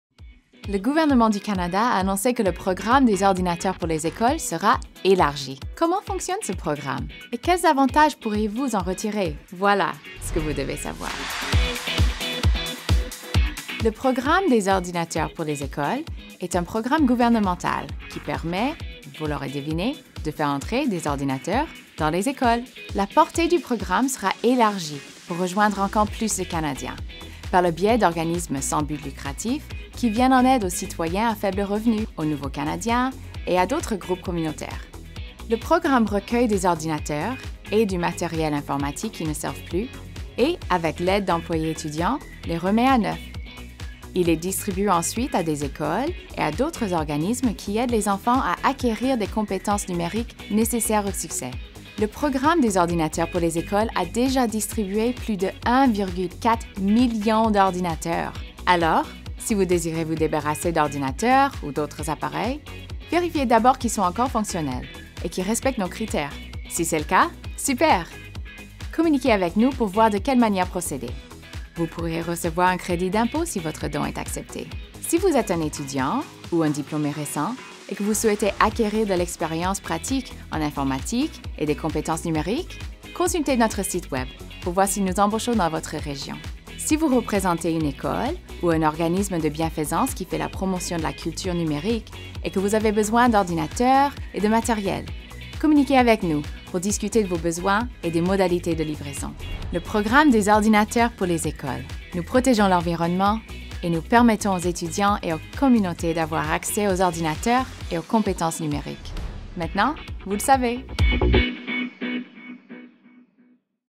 Programme des ordinateurs pour les écoles
Le gouvernement du Canada a annoncé que le Programme des ordinateurs pour les écoles (OPE) sera élargi.

Depuis 1993, le Programme OPE a distribué plus de 1,4 million d’ordinateurs remis à neuf et a employé plus de 6 000 jeunes stagiaires dans toutes les parties du pays.